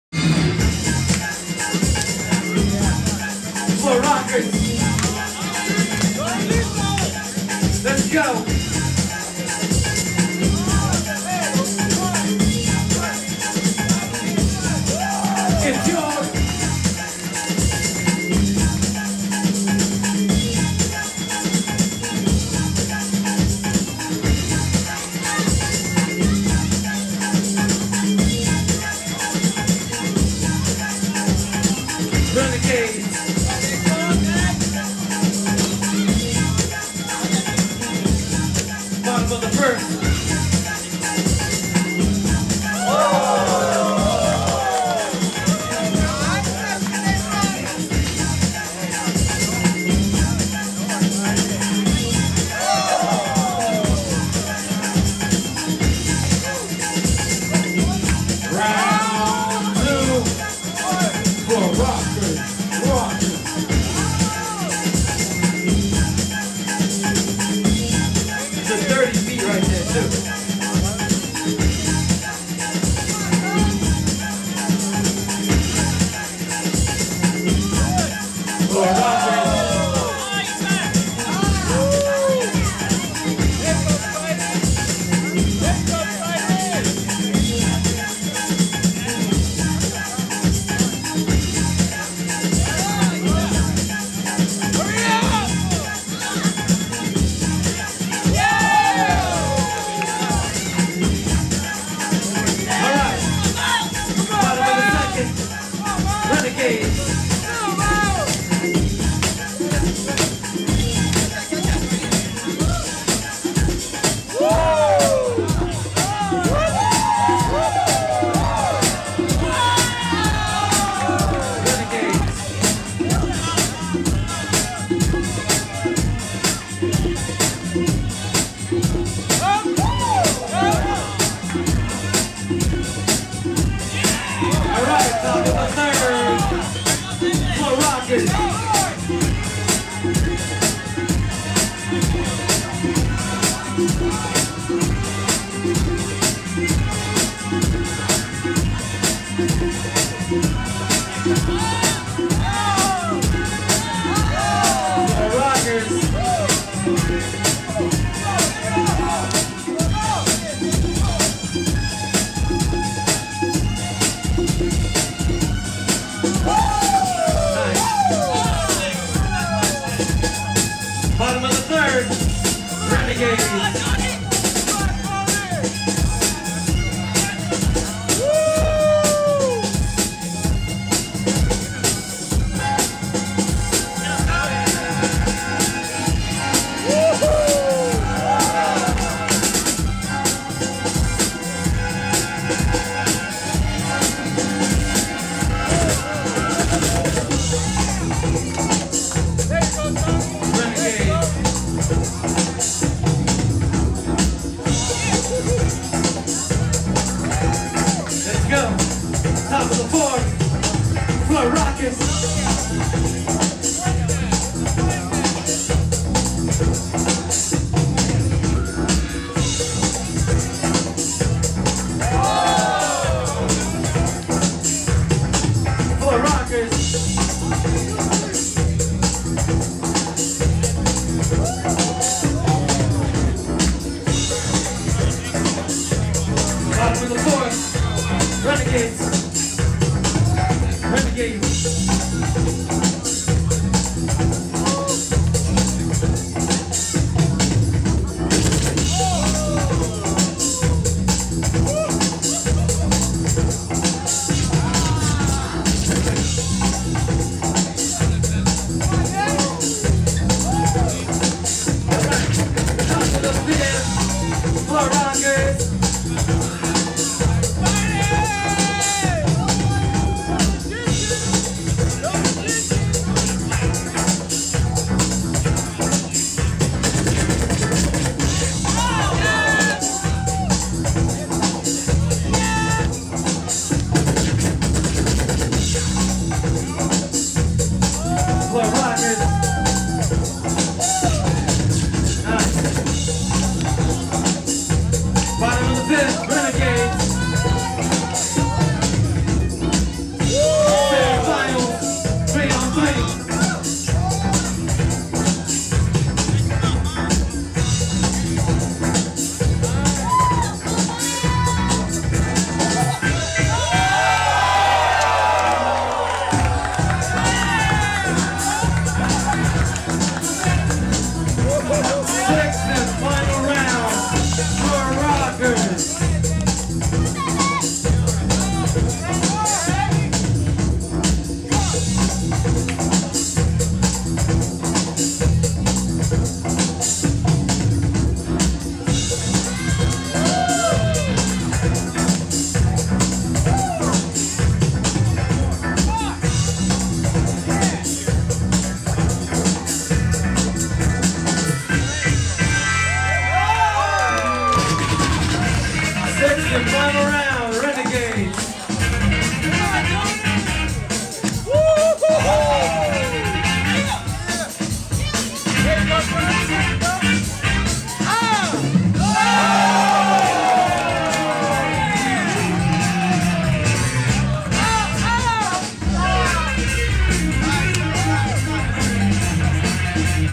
Renegades vs Floor Rockers [Round 3] BFCA2
NayTron, Tony Styles and Milestone at Battle for Cr8ive Arts Volume 2 in San Francisco CA.